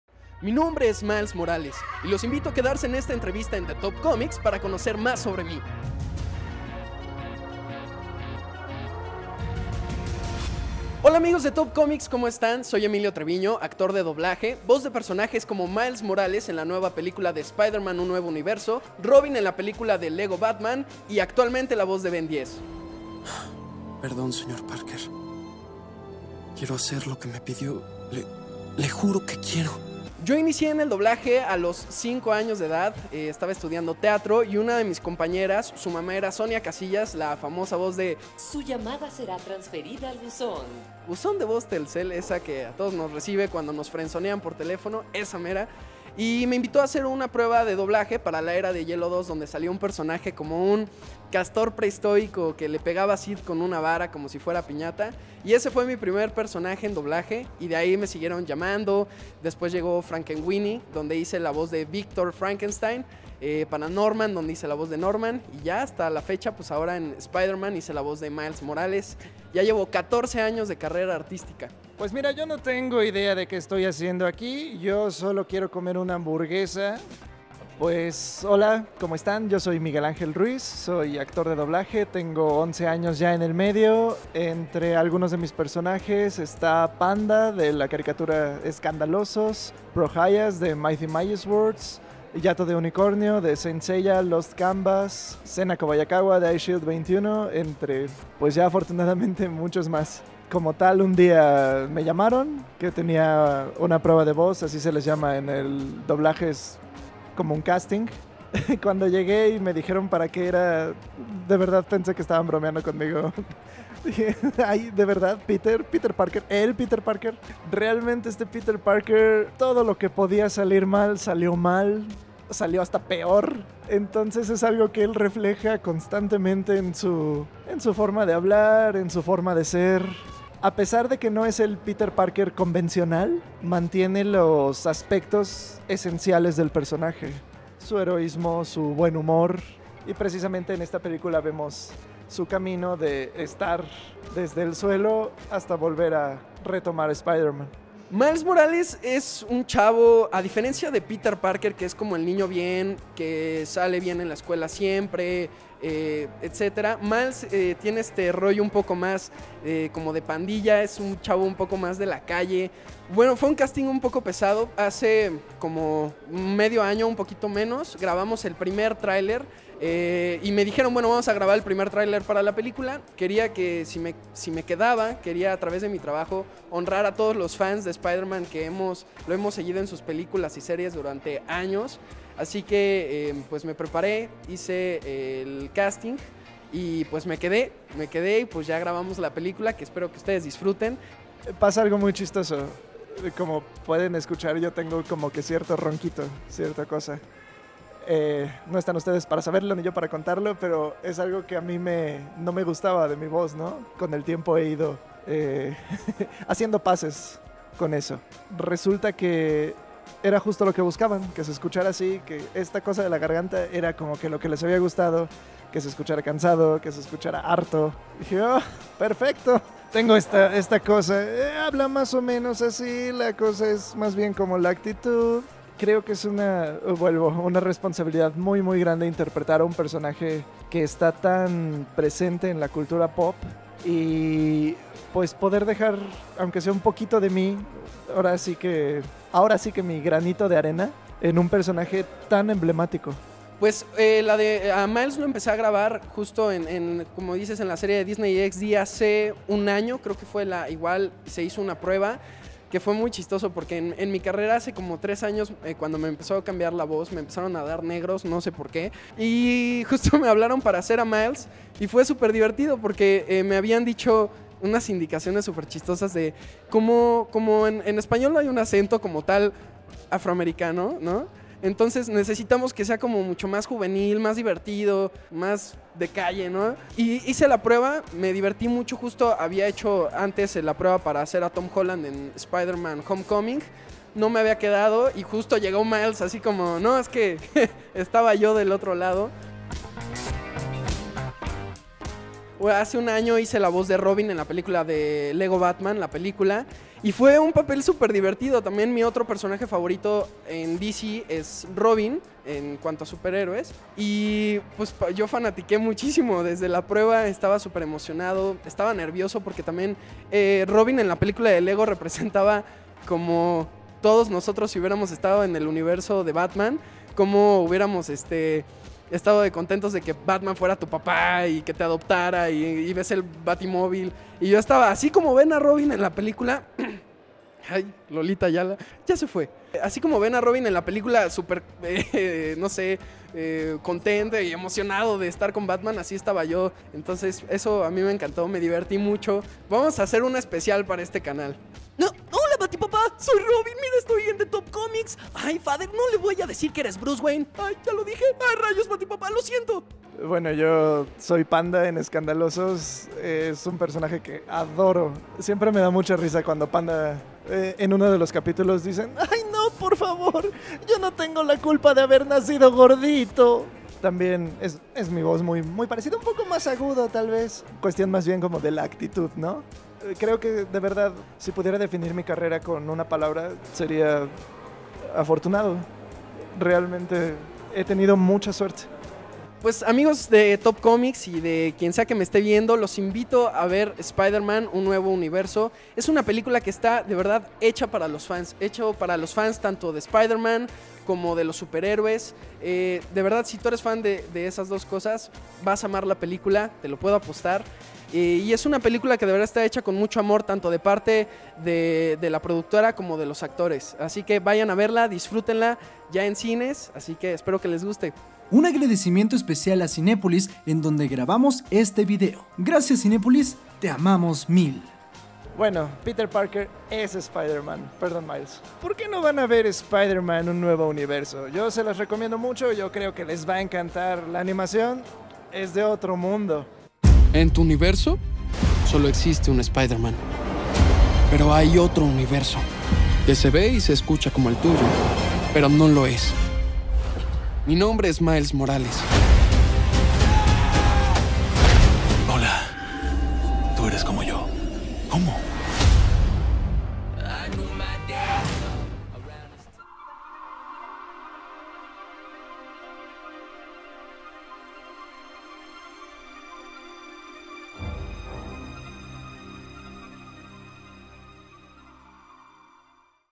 Entrevista: Voces Spider-Man Into the Spider-Verse
Conoce quién hizo el doblaje de Miles Morales y Spider-Man.